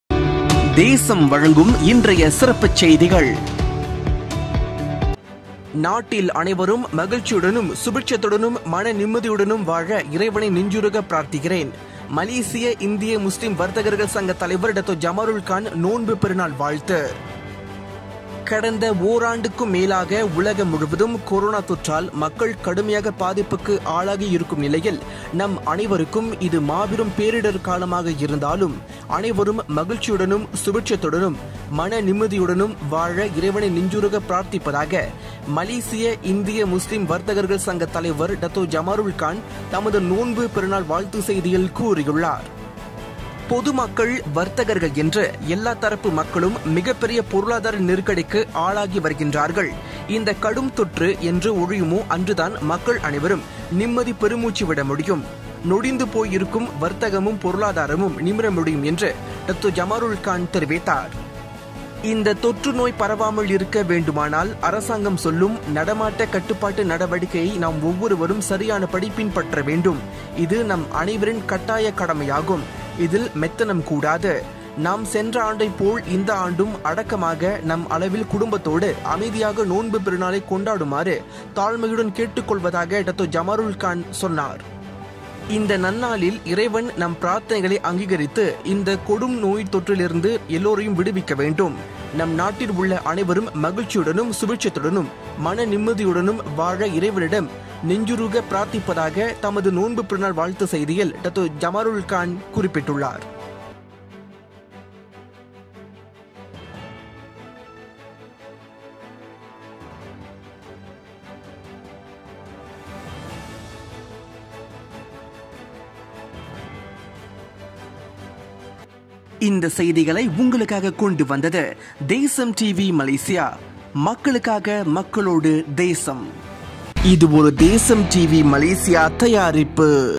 MAY 11கோவிட் 19 தொற்றில் இருந்து விடுபடுவோம்-டத்தோ ஜமருல் கான் நோன்பு பெருநாள் வாழ்த்து
MAY 11 DESAM HARI RAYA NEWS மக்கள் கோவிட் 19 தொற்றில் இருந்து விரைந்து விடுபட இறைவனைப் பிரார்த்திக்கிறேன்-டத்தோ ஜமருல் கான் நோன்பு பெருநாள் வாழ்த்து